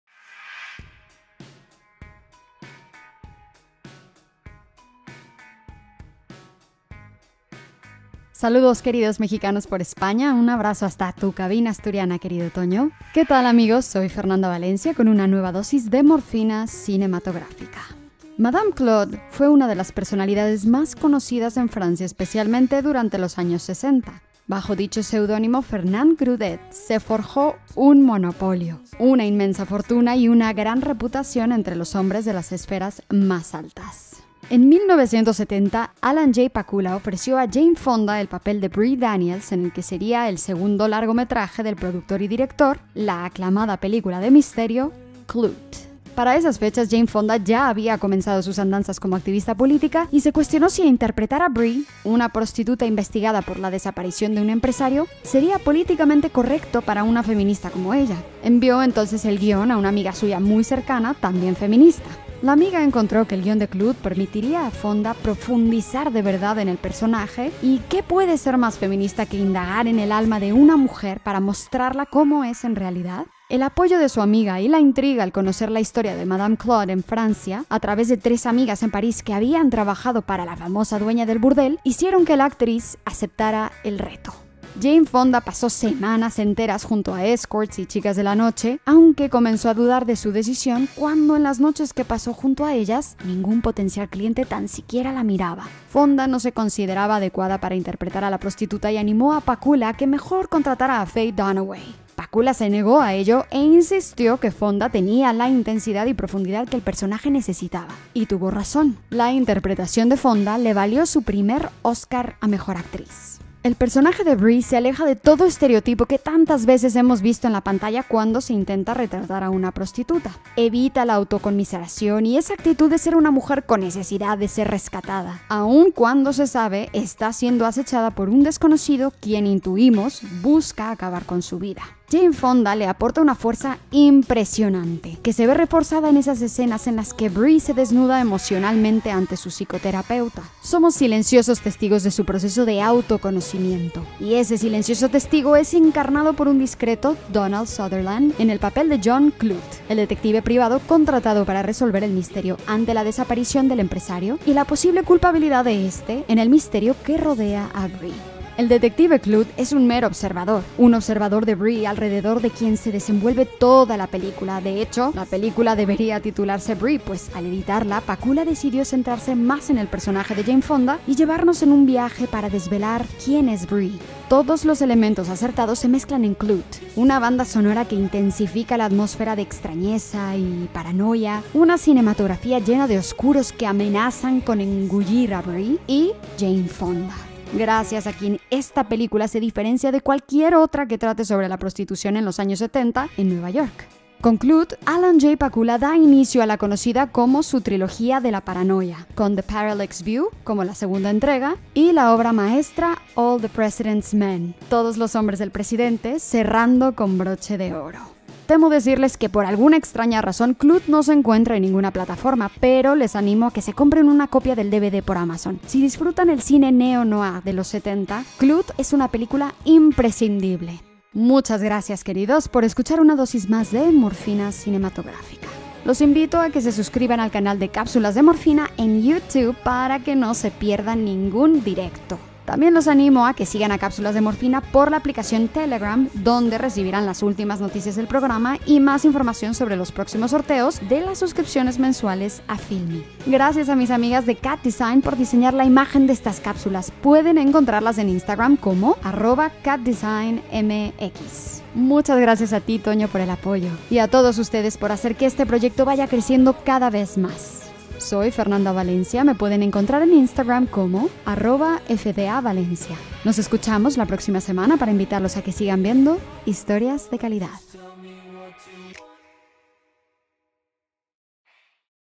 Klute 💢 Fernanda Valencia
💢 Fernanda Valencia nos comparte:
🎥  * Klute *

⏯ CÁPSULA -21/02/2021
🎤 SECCIÓN: Morfina Cinematográfica.

👁‍🗨 Actriz, escritora y productora. Nos narra «Historias de Calidad», para relajar los ánimos.